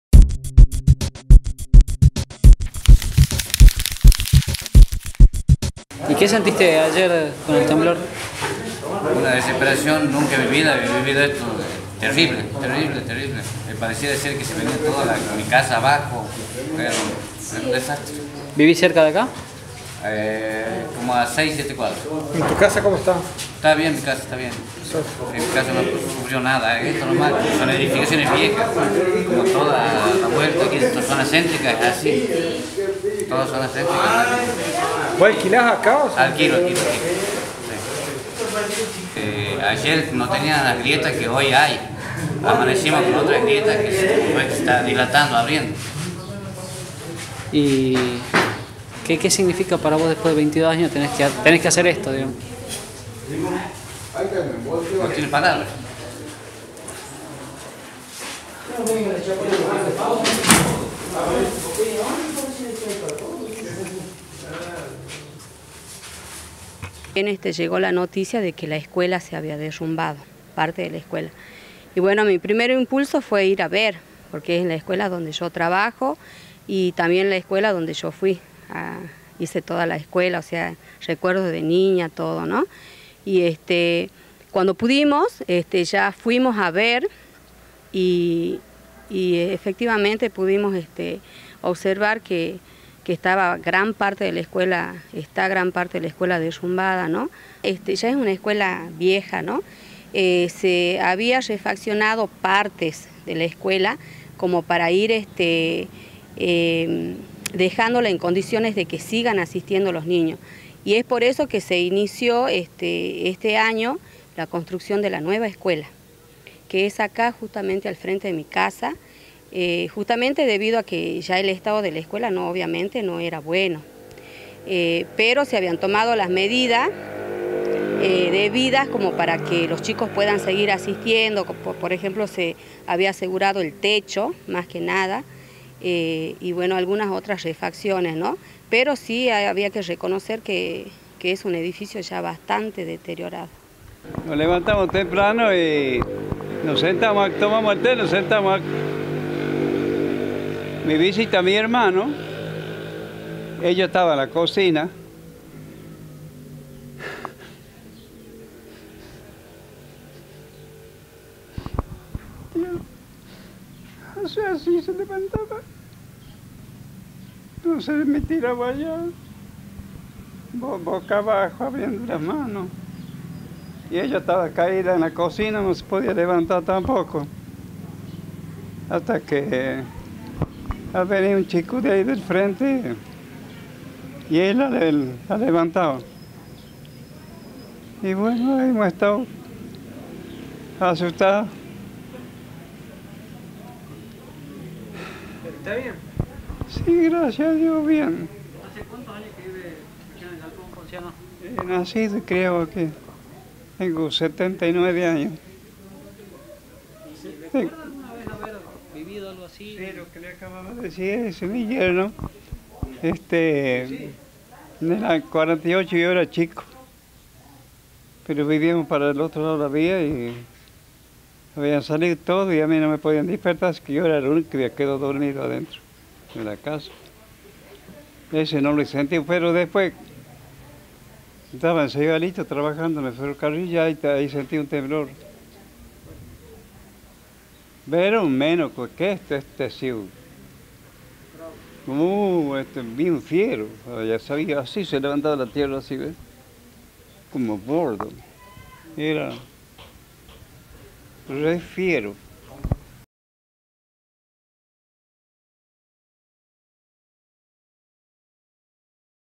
Con dramáticos testimonios, vecinos de El Galpón relataron las consecuencias del temblor
El movimiento sísmico que castigó a la localidad del sur provincial dejó familias sin viviendas, estudiantes sin escuelas y comercios cerrados.